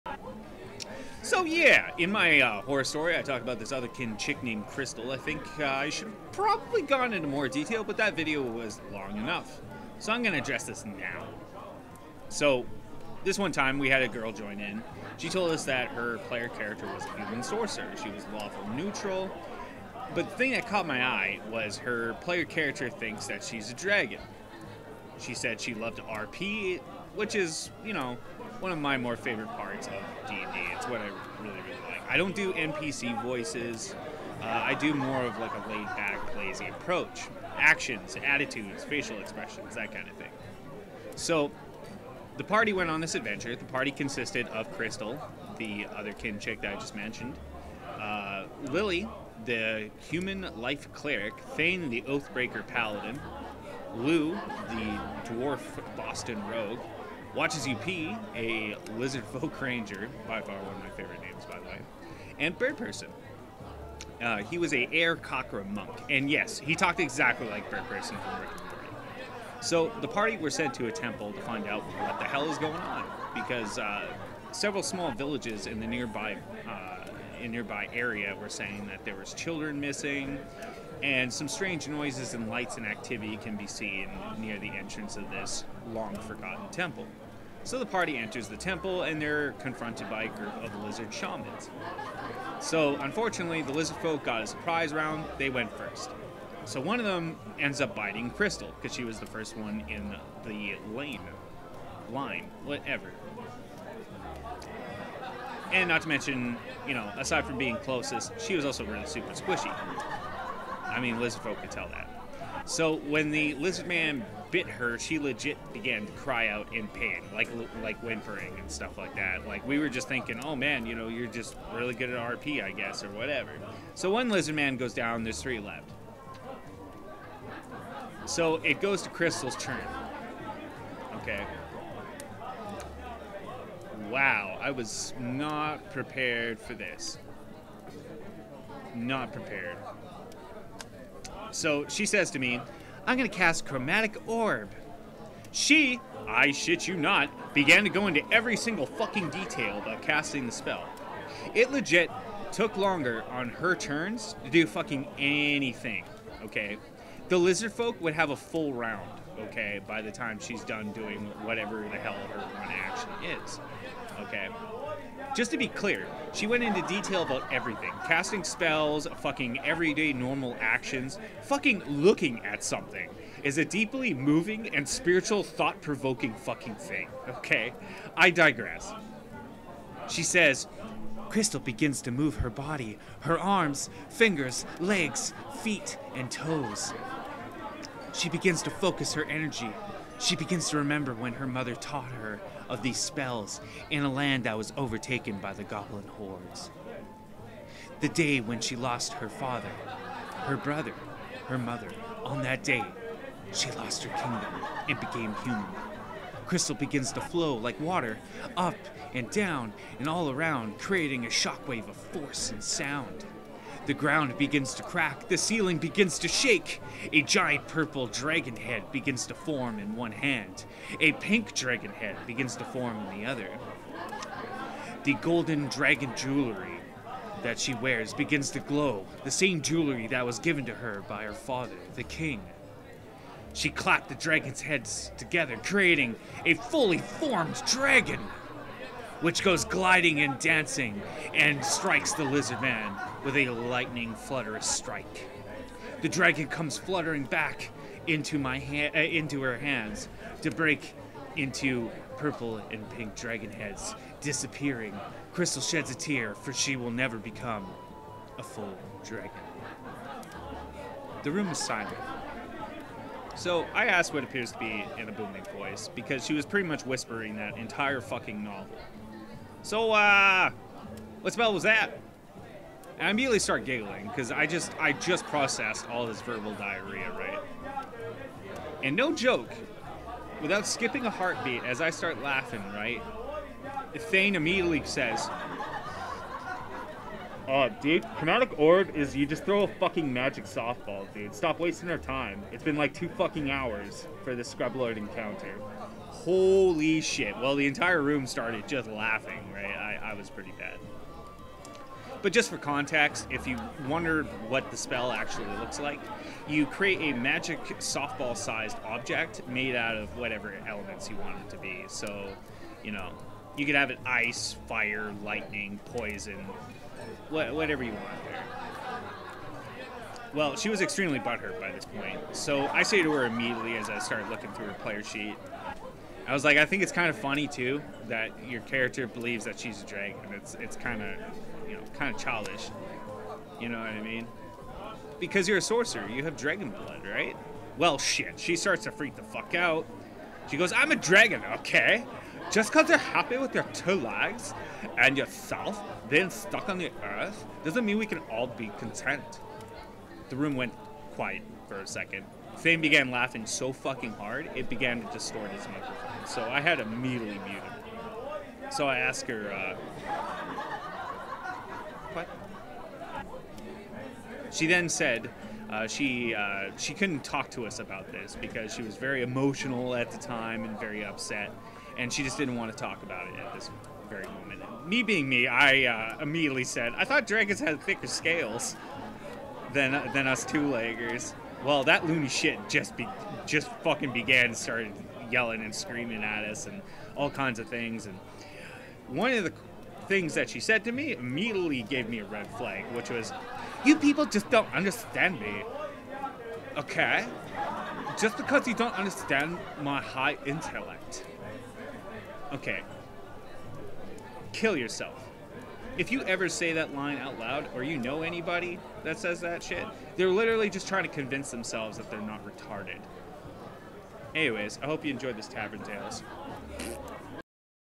Tavern Tales(D&D Story)- The "Soulbonder" Girl
Come have a sit in the "Book & Club" Tavern, As i tell a story that well question your very being, when one "Crystal" strives to be a "Dragon" in Real life. yis not Role Playing a Character, but actually being "Soulbonded" to her PC, Roll Wisdom DC against Insanity

OOOOOOOOOOH Boi